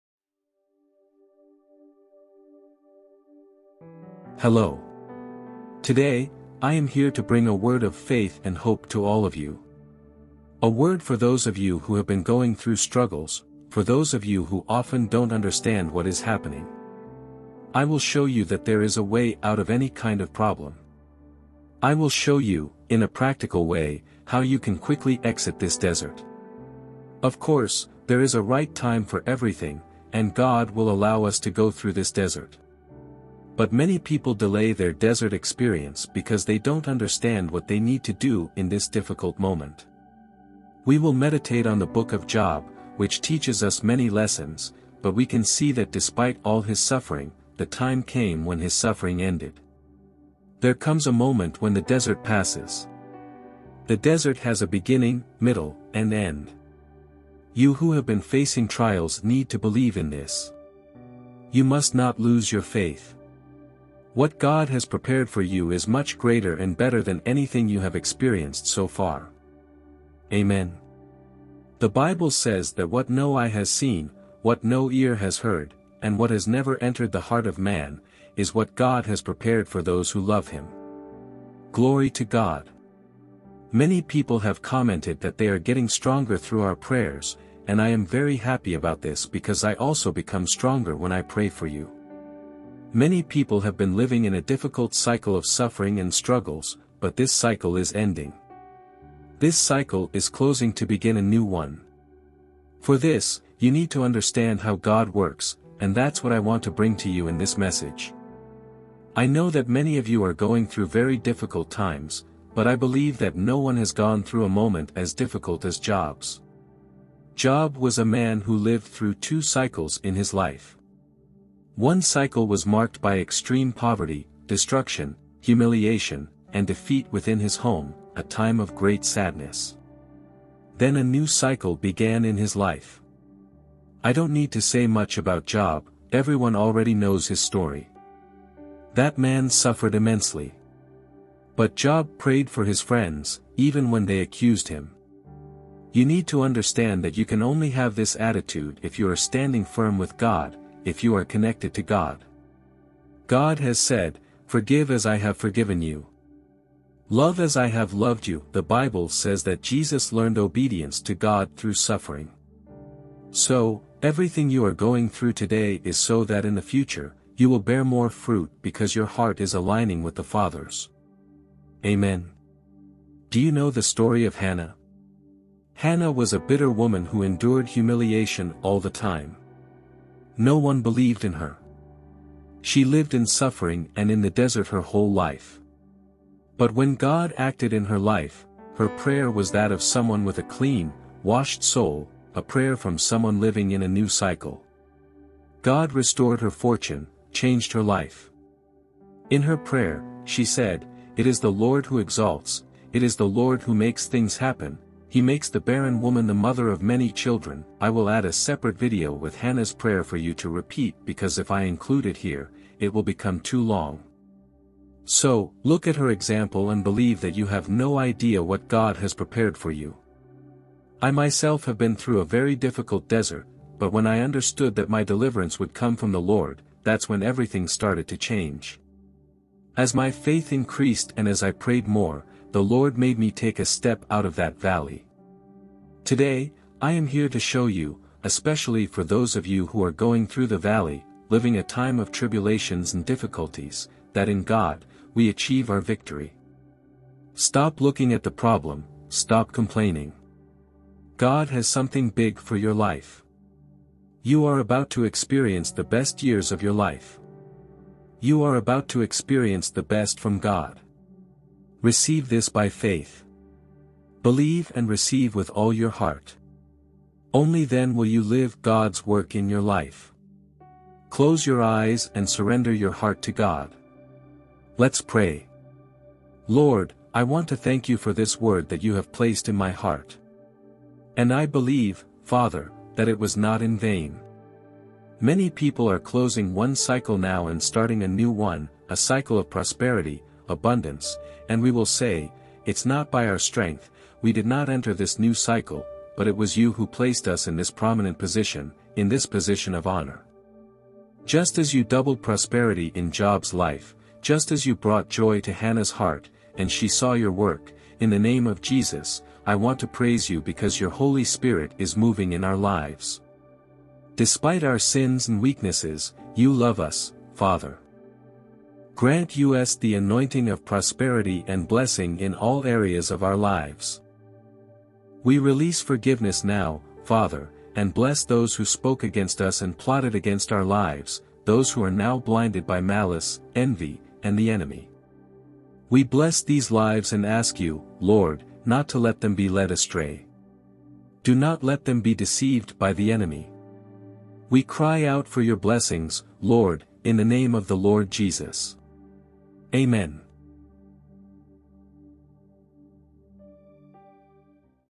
THERE ARE GOOD THINGS COMING INTO YOUR LIFE – WHAT GOD SHOWS ME IS STRONG
LET'S MEDITATE AND PRAY THE MOST POWERFUL PSALM IN THE BIBLE. OPEN YOUR HEART AND LET'S PRAY WITH ALL OUR FAITH IN GOD.
GREAT BLESSINGS WILL BE POURED INTO YOUR LIFE.
IN LORD JESUS' NAME.